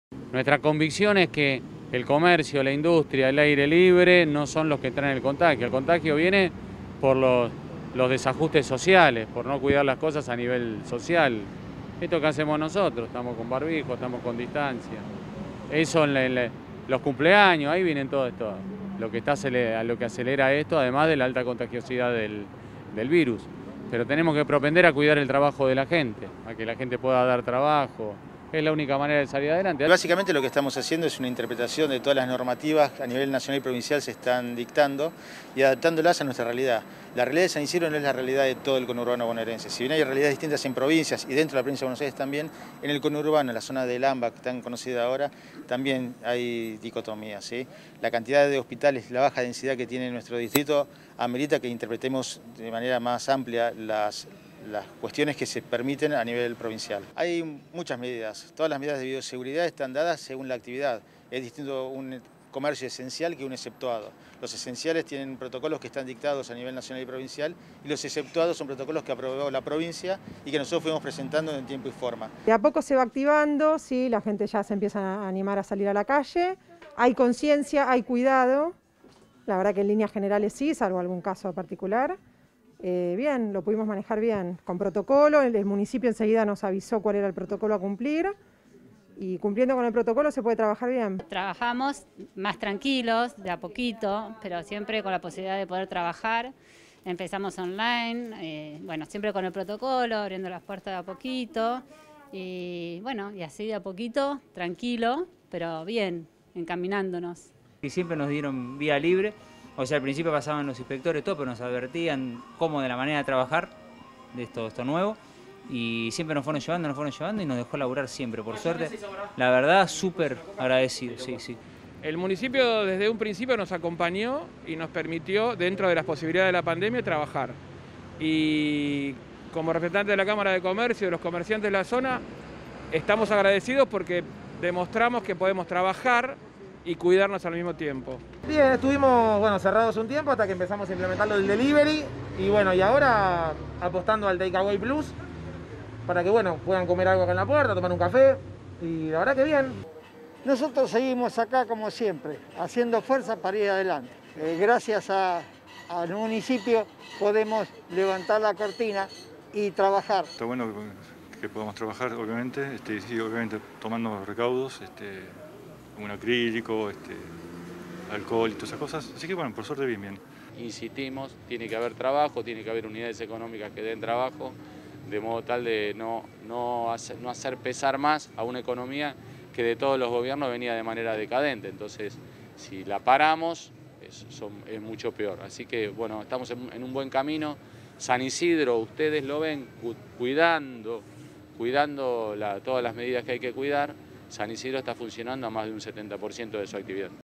San Isidro: en pandemia el 90 % de los comercios pudo trabajar con protocolos
El intendente Gustavo Posse recorrió los centros comerciales de todas las localidades. Los comerciantes manifestaron su satisfacción por las progresivas habilitaciones desde que empezó la pandemia por coronavirus.

(San Isidro, 26 de agosto de 2020). “San Isidro fue uno de los pocos municipios que bajo estrictos protocolos sanitarios nos dejó trabajar. Tengo colegas de otros distritos que hace cinco meses no pueden hacerlo. Tener la oportunidad de poder abrir el local en esta situación tan difícil fue muy bueno para nosotros”, opinó Mario Vargas, dueño de una dietética frente a la Estación de Villa Adelina.

La experiencia de este comerciante se replicó en el 90 por ciento de los comercios de todo el Partido de San Isidro. Los locales funcionaron aplicando los protocolos que el Municipio dispuso desde el comienzo de la cuarentena y que permitió que la actividad económica pudiera continuar.

“Gracias al Municipio podemos levantar la cortina y trabajar. Estoy muy agradecido”, expresó, desde Beccar, Fioravante la Grotteria, dueño de un local de llaves sobre Ayacucho.

Las largas recorridas local por local comenzaron en San Isidro, donde el intendente Gustavo Posse caminó por Belgrano desde 25 de mayo hasta Av. Centenario. “La idea es que cada vez haya mayor actividad económica. Porque nuestra convicción es que el comercio, la industria y el aire libre no son los factores que traen el contagio, vienen por otros descuidos sociales. Realicé extensas recorridas por los centros comerciales a cielo abierto de todas las localidades y vi muy contentos a los comerciantes”, contó Posse.

Cintia Sánchez, del local “Dulces y algo más”, en Rivadavia al 300, sostuvo: “Veo al menos en mi zona que la gente es consciente sobre la importancia del barbijo y el distanciamiento. Con el protocolo que nos bajaron desde el Municipio pudimos trabajar bien y de a poco se van activando las ventas”.

Sobre 9 de julio al 300, María José Munne, de “Naranjo” agradeció al intendente la posibilidad de haber podido abrir su local. “Lentamente, nos vamos encaminando”, señaló la dueña del local de ropa y calzado para bebés.

La recorrida del intendente culminó en uno de los negocios emblemáticos de la calle Belgrano con más de 60 años de vida: la panchería “Coquito”. Adrian Rubini, uno de los dueños, remató: “Aplicando el protocolo sanitario se puede trabajar perfectamente. Estamos súper agradecidos con el Municipio. Dea poco sube el trabajo”.

Protocolos para industrias, comercios, profesiones, obras particulares, el “take away plus “, la peatonalización de las calles Alvear y Belgrano y, recientemente, el regreso de los deportes individuales y por ende la actividad económica que generan, son las medidas más aplaudidas. “Lo que necesitamos es que nos dejen trabajar, con protocolos, pero no dejar de trabajar”, dicen los comerciantes.

Posse destacó que todas las habilitaciones se hicieron a conciencia, con protocolos que legal y sanitariamente son “impecables y respetuosos de la normativa vigente” y que fueron posibles “gracias al robusto sistema de salud, enteramente municipal, que nos da la tranquilidad de poder atender adecuadamente la pandemia”